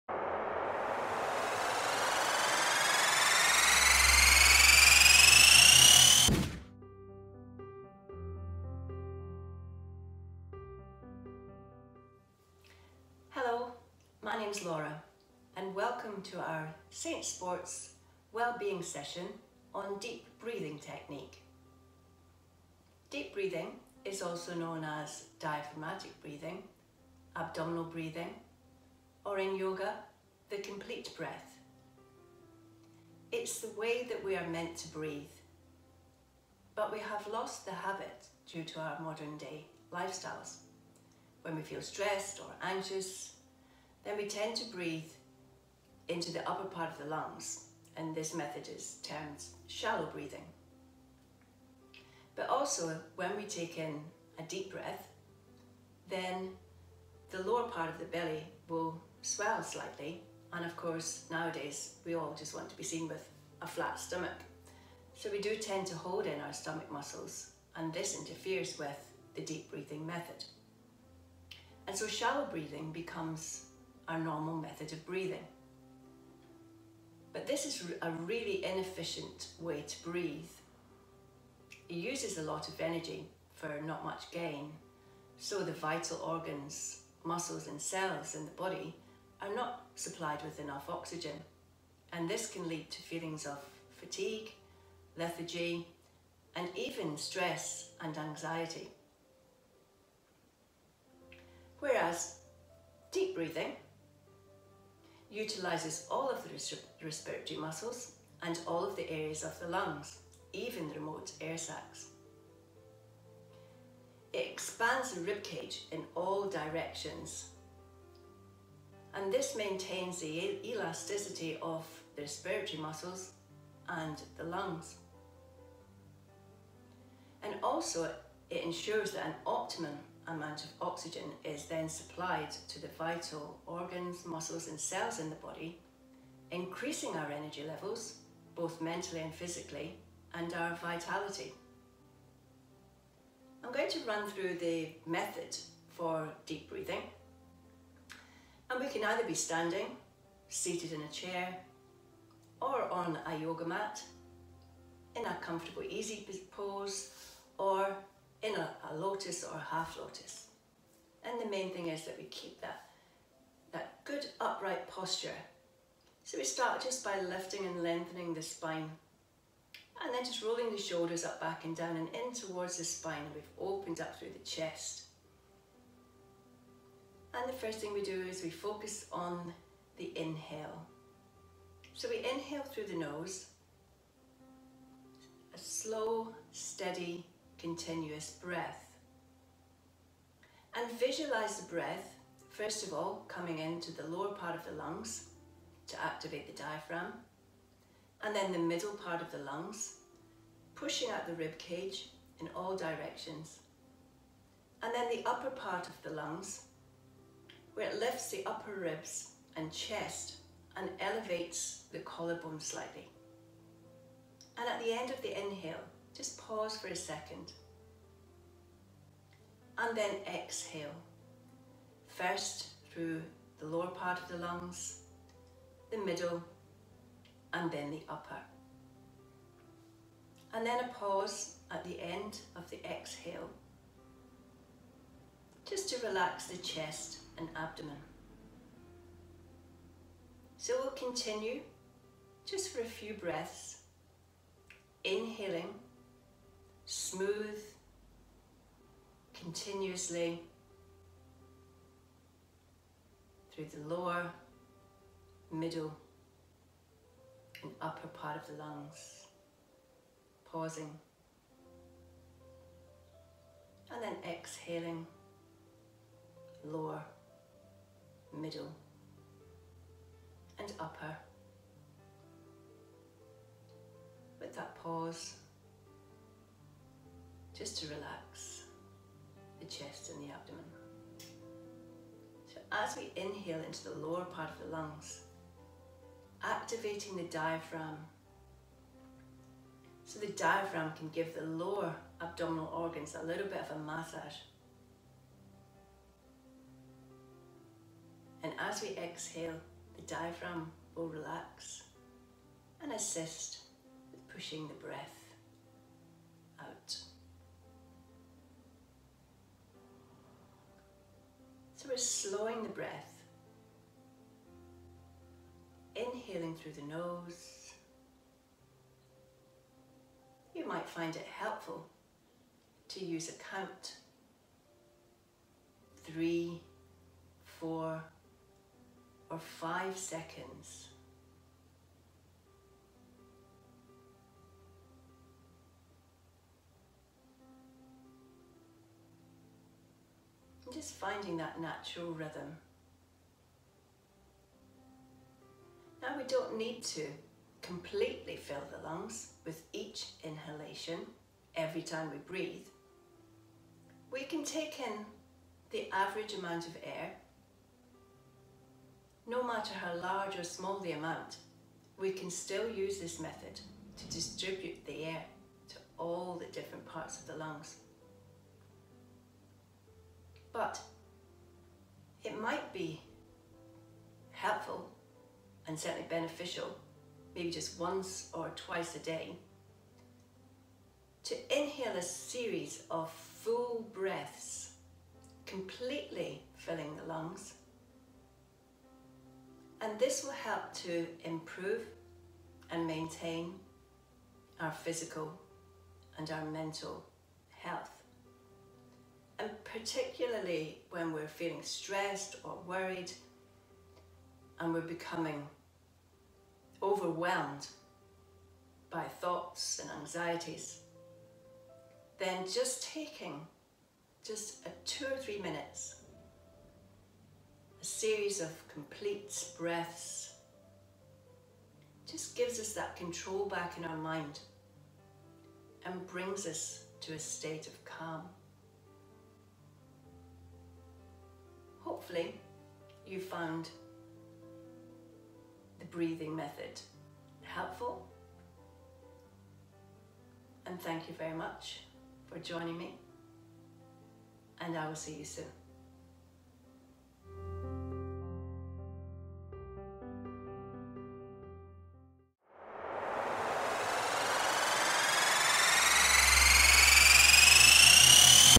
Stay Home | Stay FIT: DEEP BREATHING WITH LAURA
Laura takes you through a deep breathing session which you can perform in your living room, garden or bedroom.

Stay Home | Stay FIT